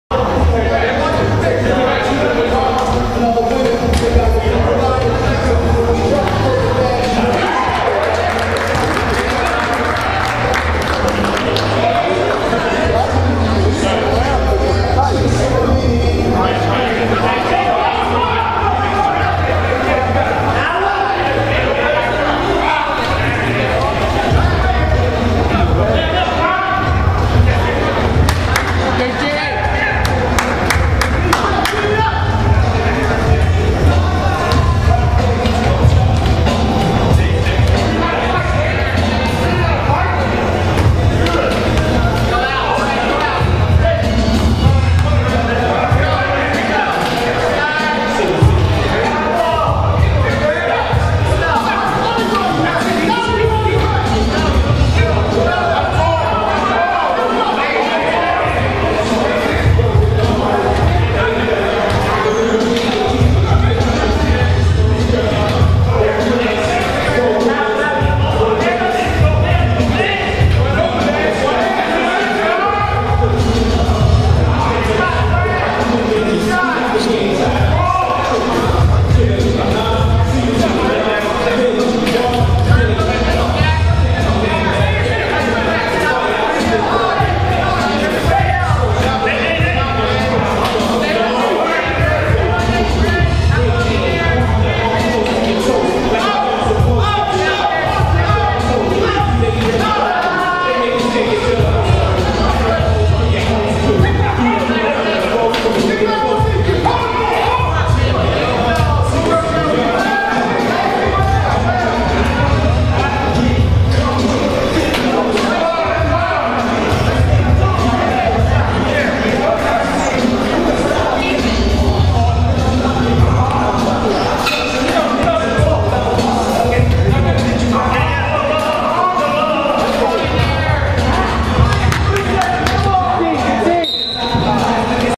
2nd Annual Wheelchair Basketball Tournament 2016 Game Time
Watch the competitive nature of Vash Turner and his wheelchair basketball league as they face off against their wheelchair competitors  the Sixers. Both teams played hard and really gave each other a hard way to go trying to score points. Anyone watching this sport would have to appreciate the players for what they do on and off the court.